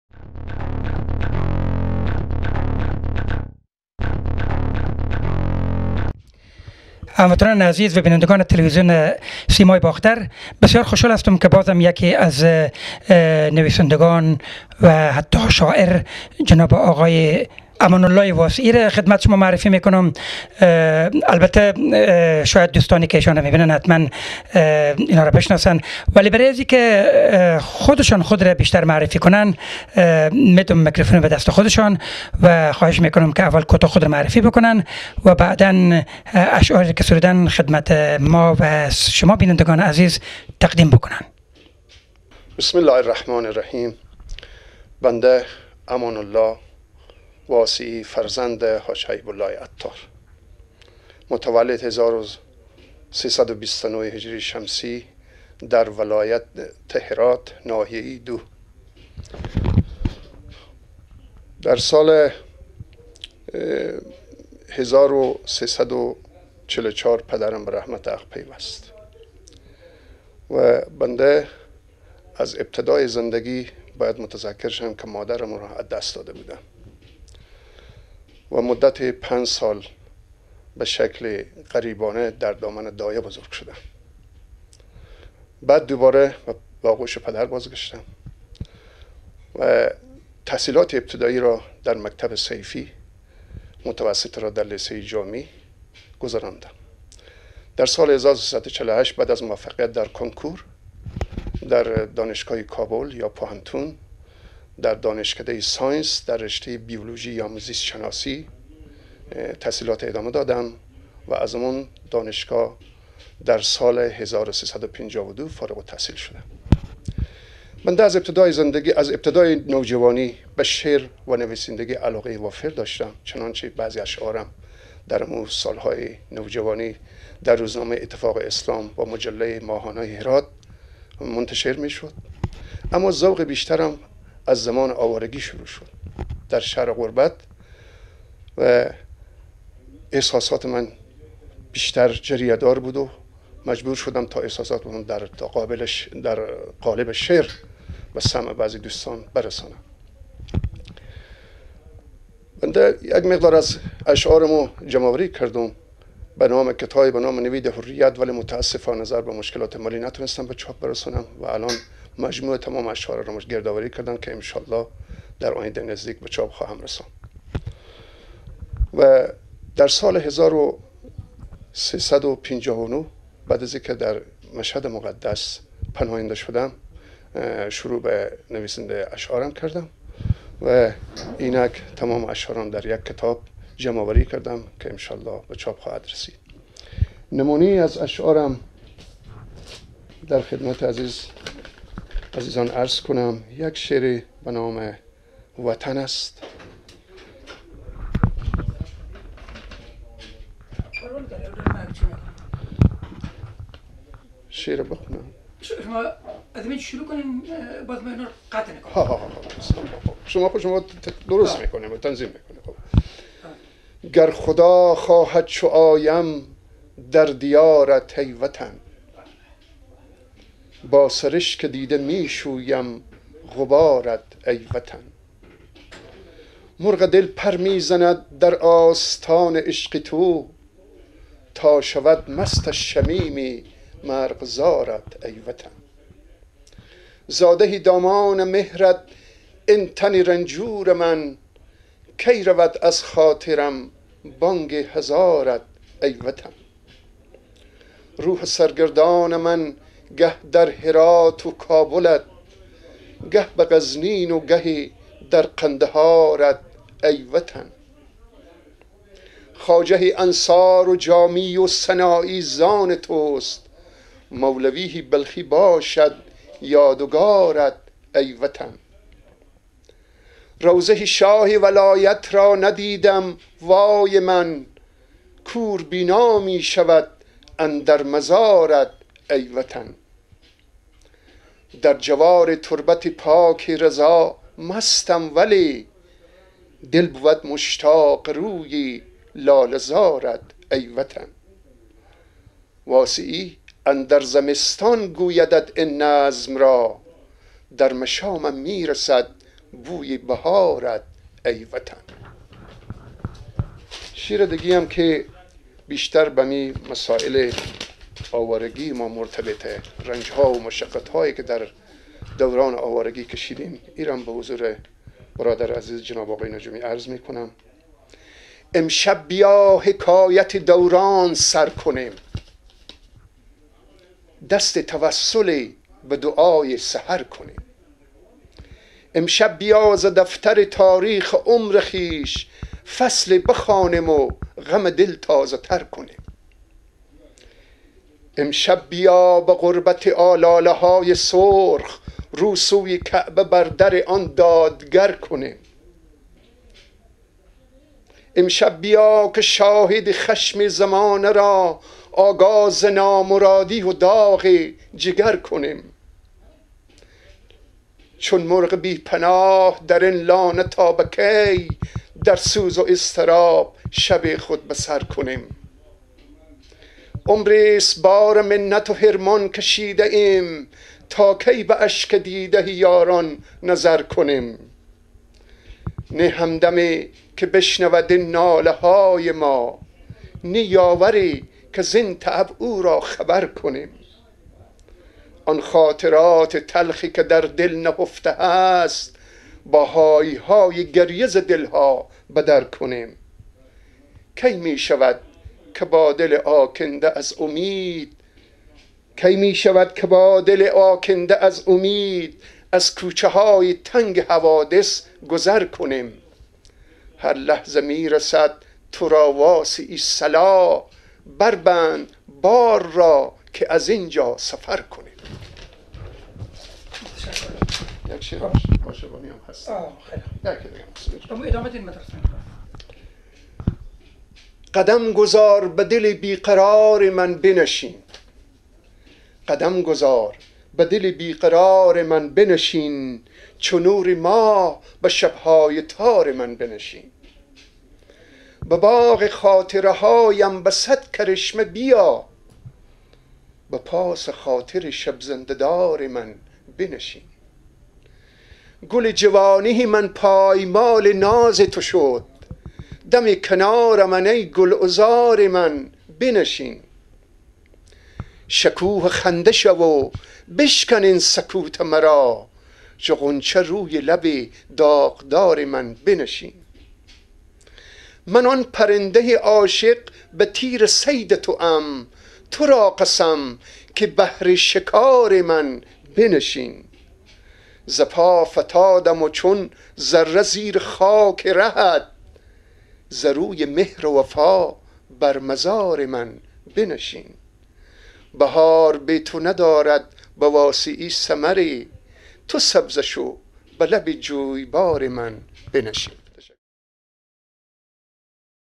TV Bachtar Berlin اشعارآقای امان واسع
آقای امان واسع متولد سال ۱۳۲۹ هرات که تحصیلات اش را در رشته شیمی دردانشگاه کابل به پایان رسانده است و علاقه فراوان به شعر و ادبیات دارد . او اشعار زیادی را سروده ، که به زودی به نشر خواهد رسید نمونه از اشعارش را دراین فیلم کوتاه میشنوید.